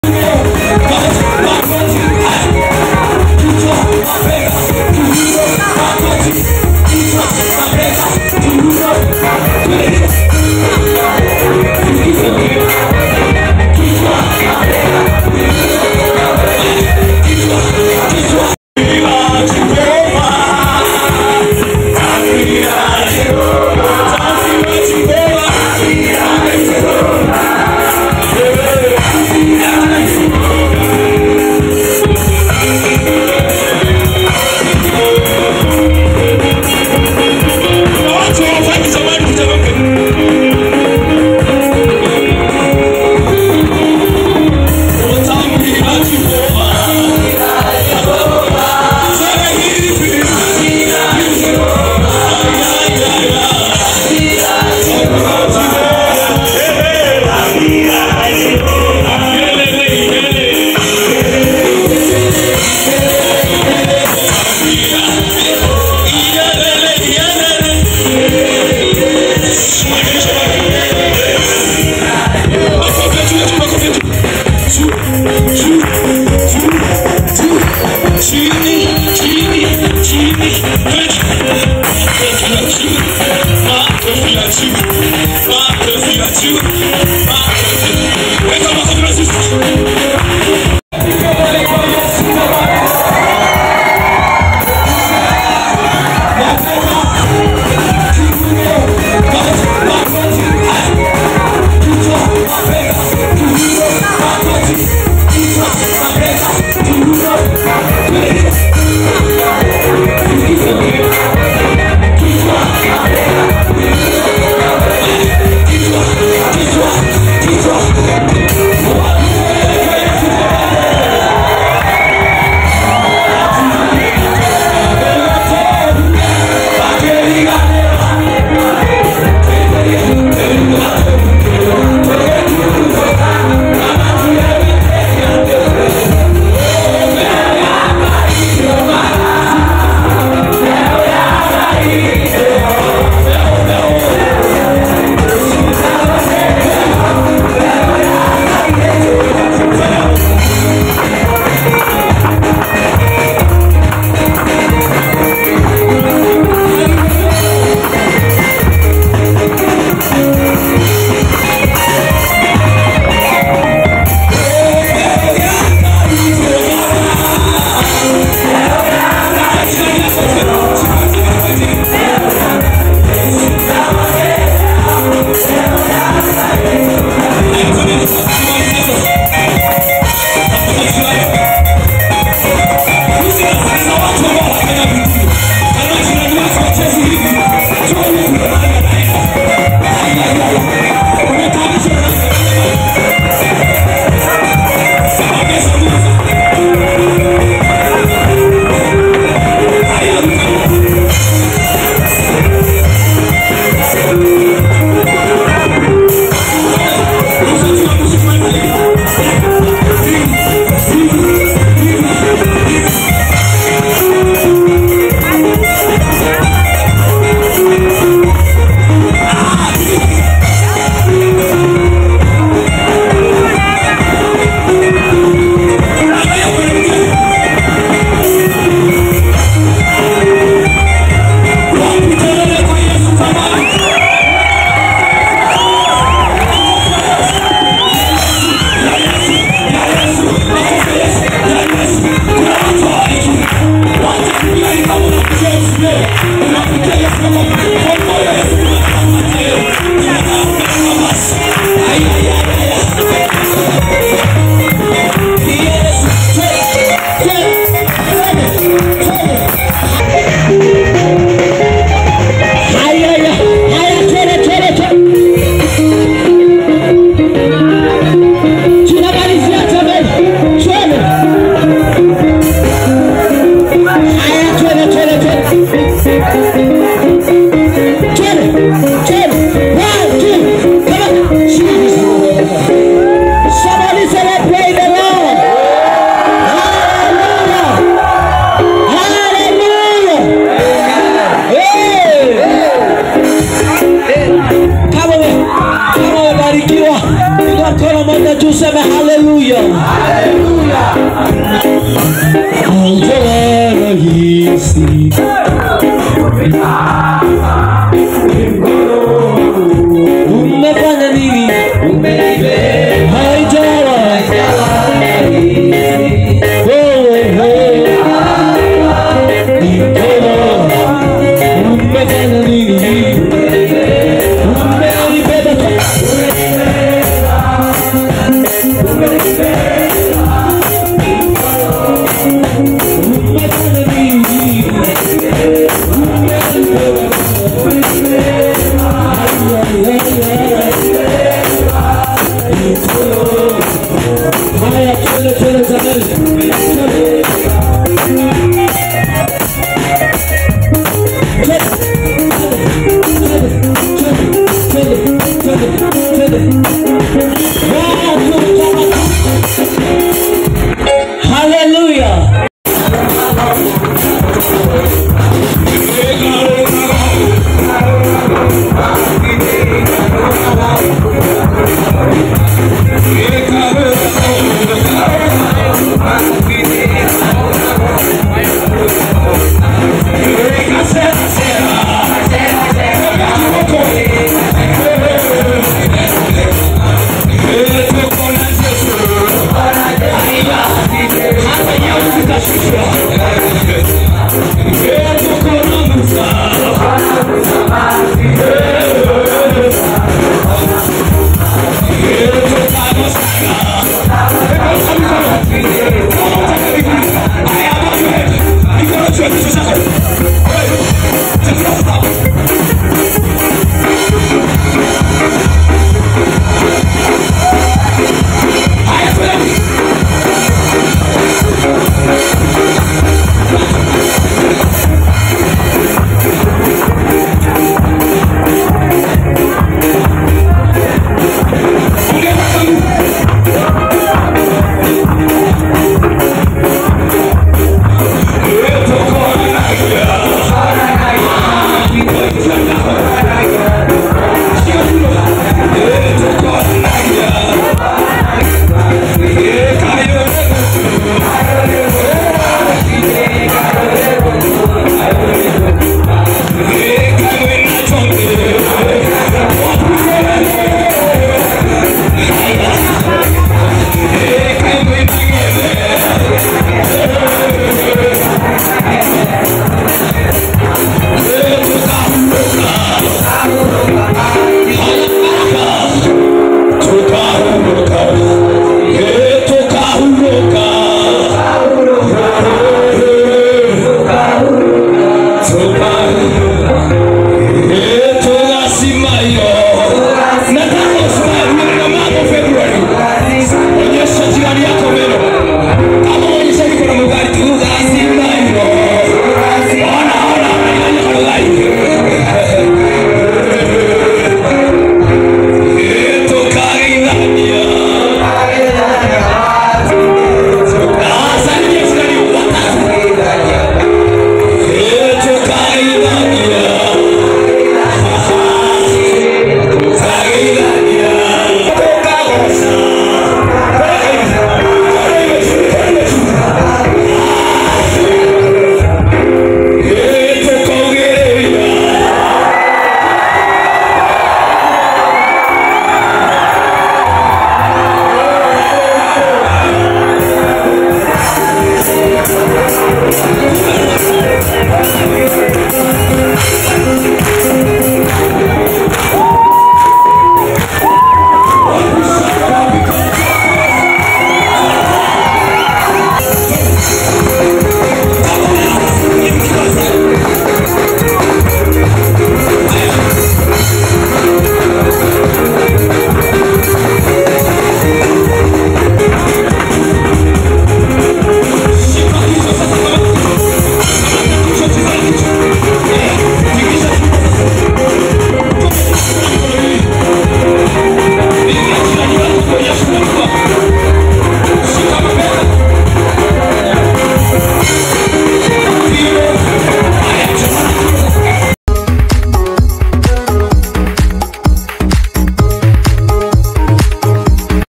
Inside Kamuiru Boys' High School Powerful Sunday Service | Maina J.
Students of Kamuiru Boys' High School, praise powerfully during Sunday Service